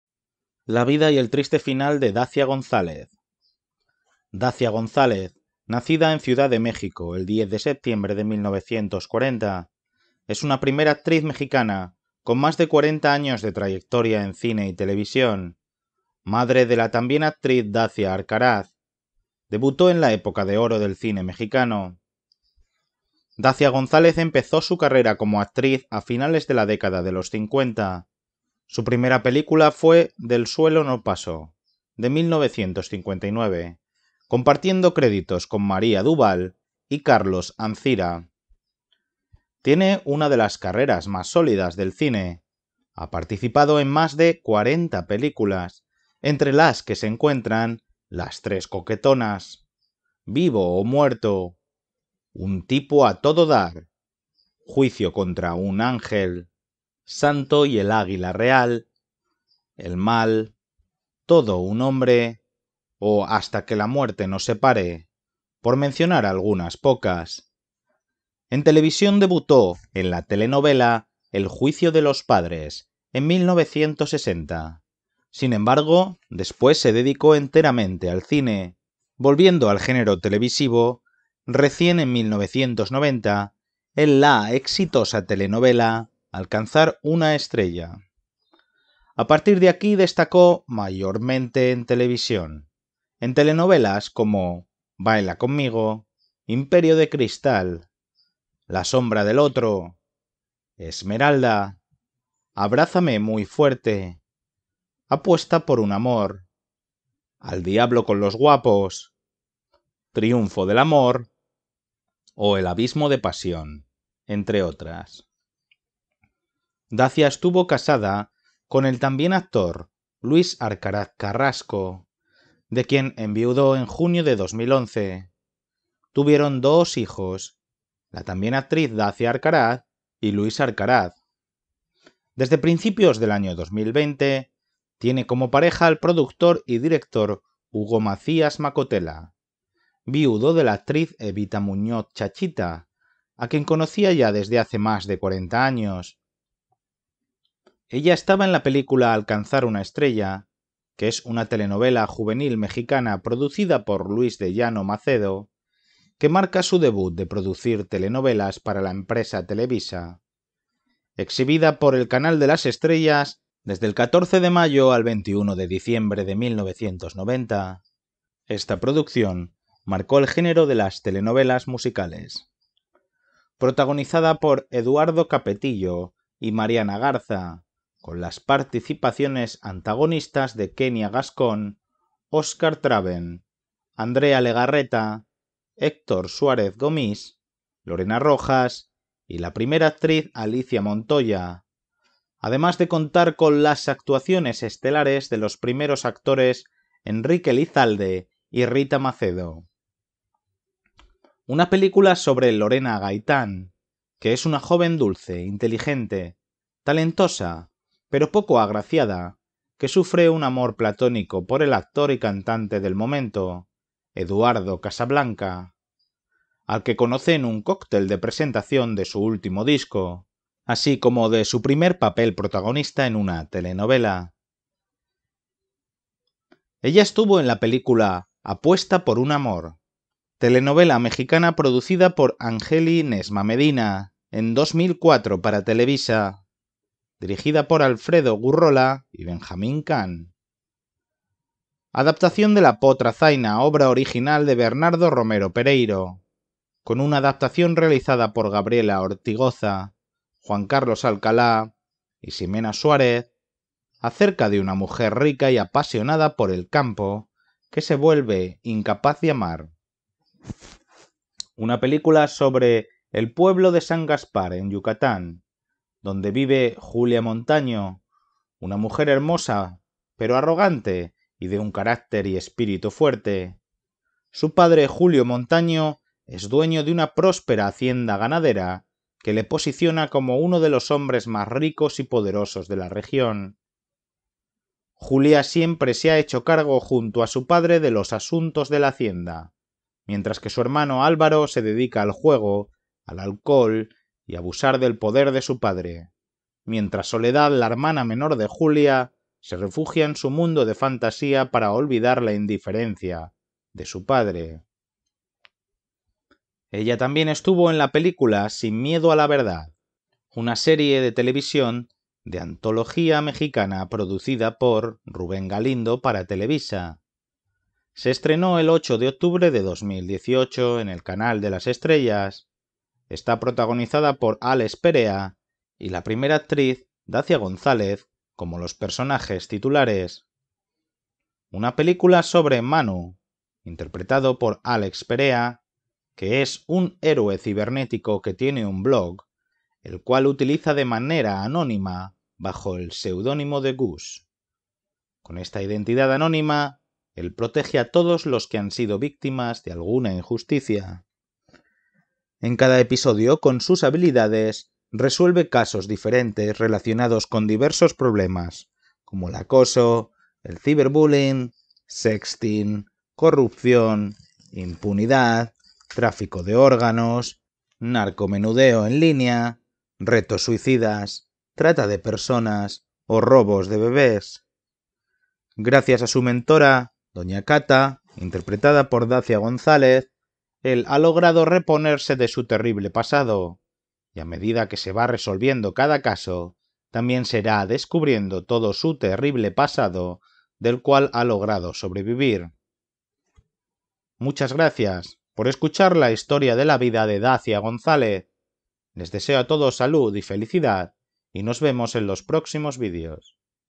La Vida y El Triste Final de Dacia González
Mini biografía de la vida del Dacia González. Dacia González (Ciudad de México, 10 de septiembre de 1940) es una primera actriz mexicana con más de cuarenta años de trayectoria en cine y televisión. Madre de la también actriz Dacia Arcaraz. Debutó en la Época de Oro del cine mexicano.